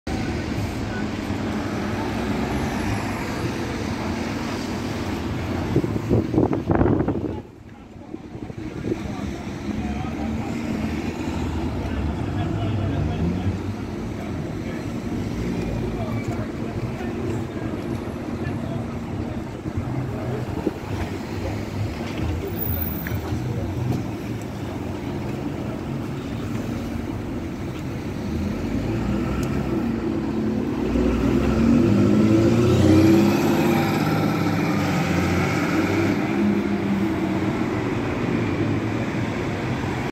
Low-ish Floor Running Day - Metroline - TA ALX400 - TA648 - LK05GGP - at East Grinstead - 16/07/2023
The North London Collection's 2nd Event - The Low-ish Floor Running Day Event - Sunday 16th July 2023.
Rear engined buses from the 60s till the 00s running up and down the lanes between Godstone and East Grinstead, hosted by TNLC and BBPG.